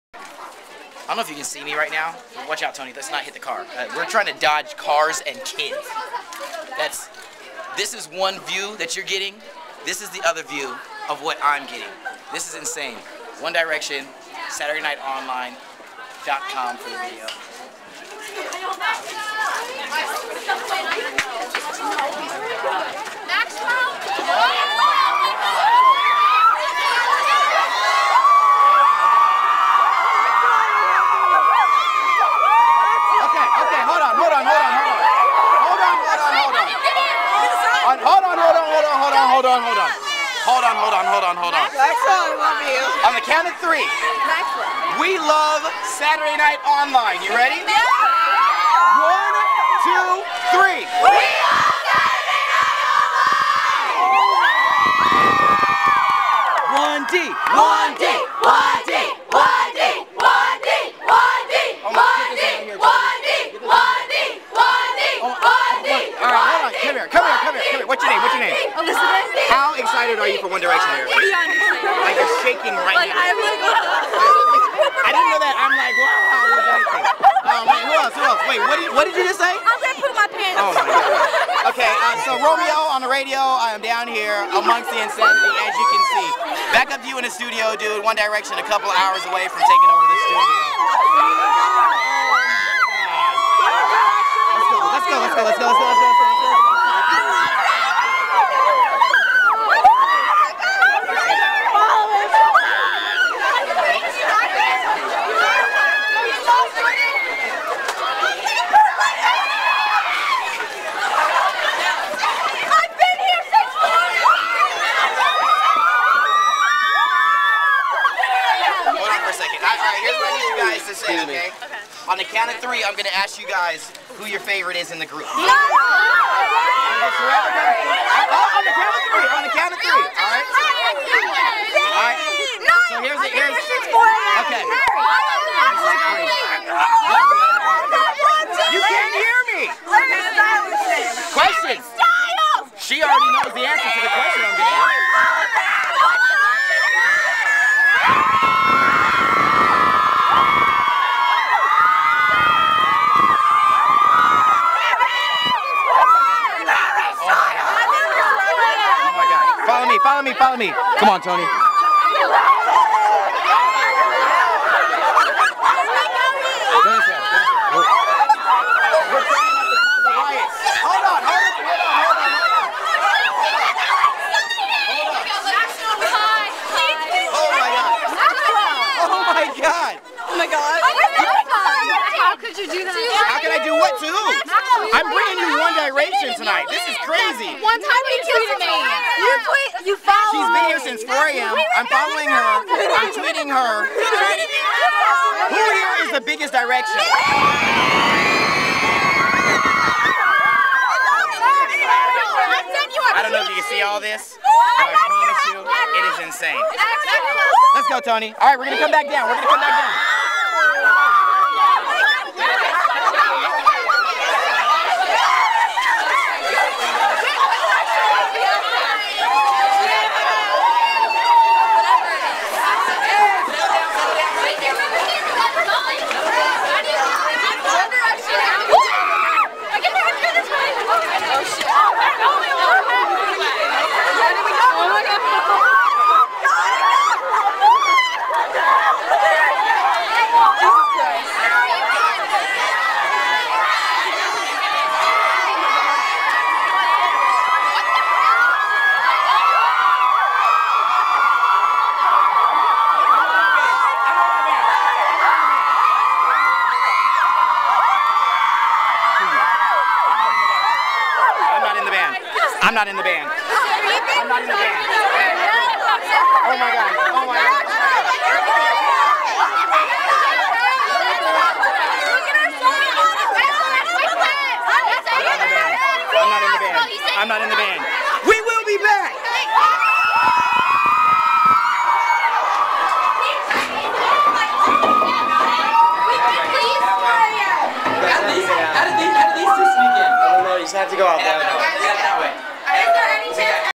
One Direction Fans Outside at Saturday Night Online
Here's what's goin down here Outside the Saturday Night Online Studios...waiting for One Direction to arrive!